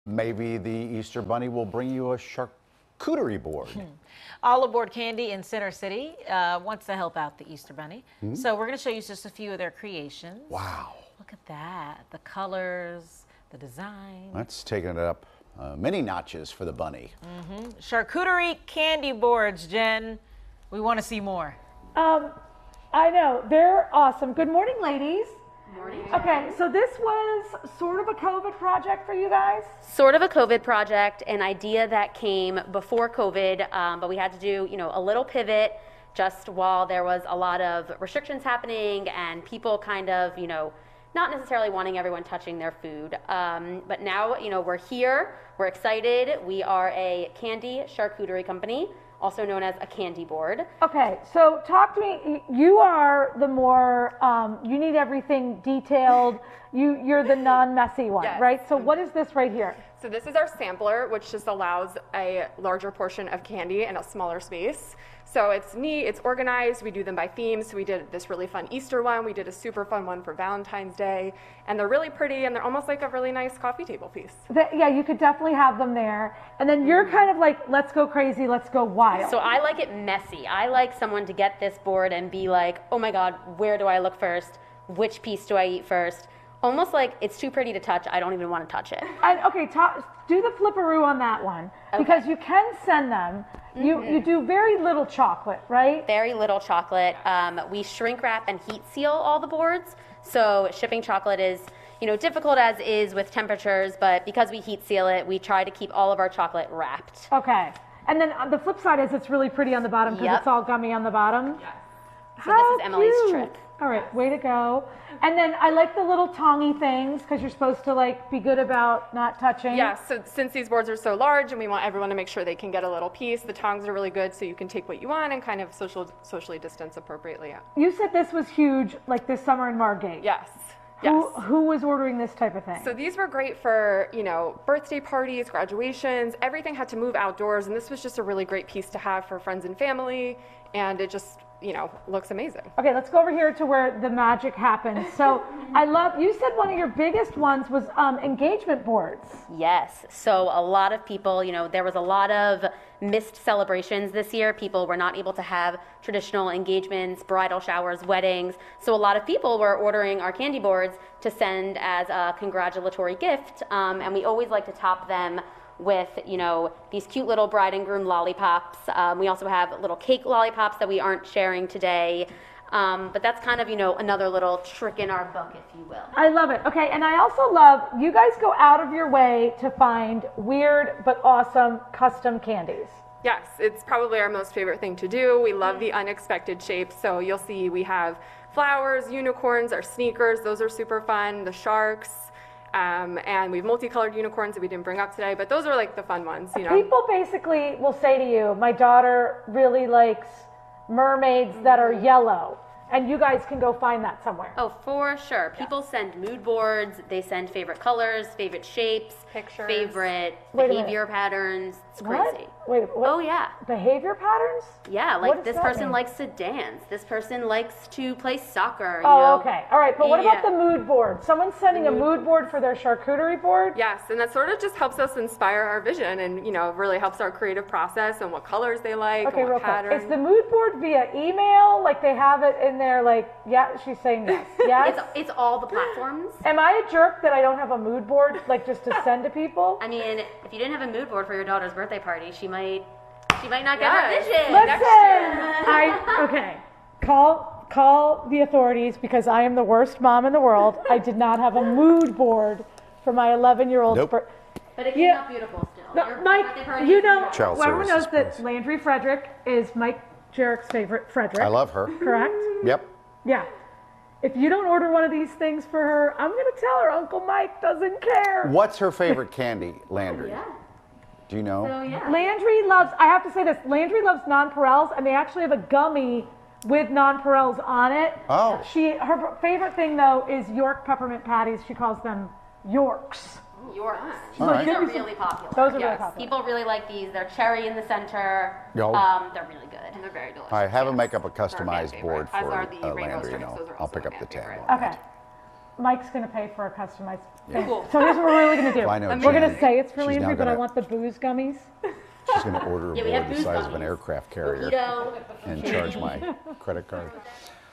Candy charcuterie boards are the newest sweet treat trend
Jenn Frederick visits All Aboard Candy in Center City, Philadelphia where they're creating charcuterie boards made of candy to get ready for Easter.